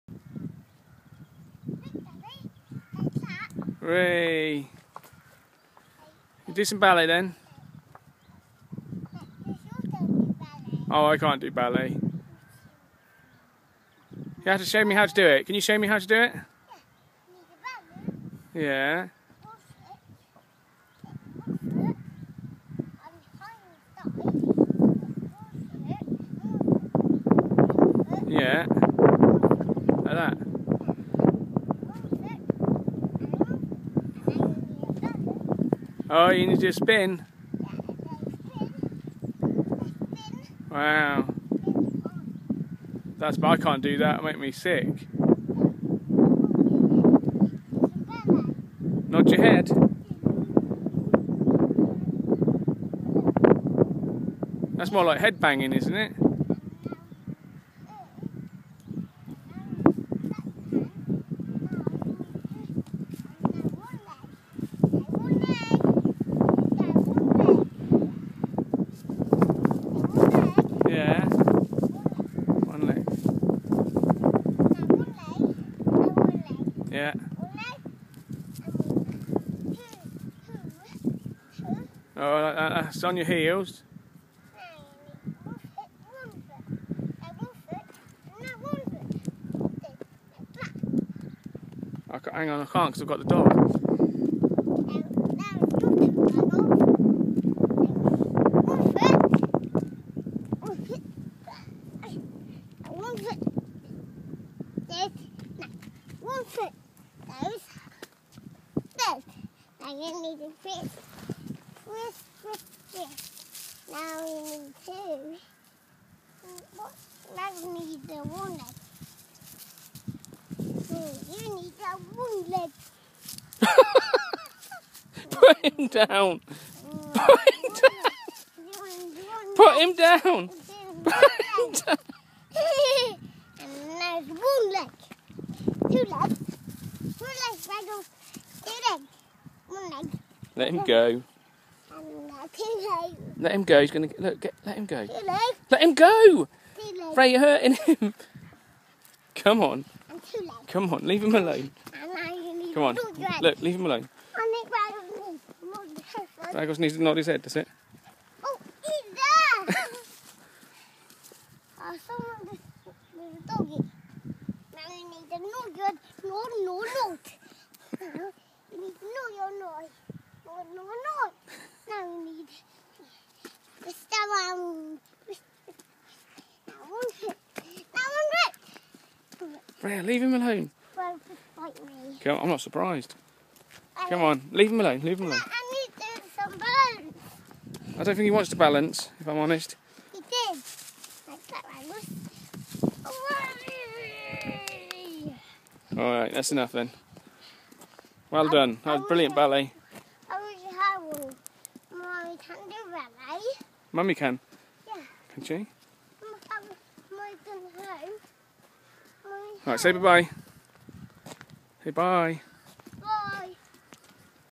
The dog has to dance!
While out walking the dog my two year old daughter (who has never had a dancing lesson in her life) decided to show me how to do ballet, later in the video you will see that she decided that the dog also ha to do some ballet with hilarious consequences!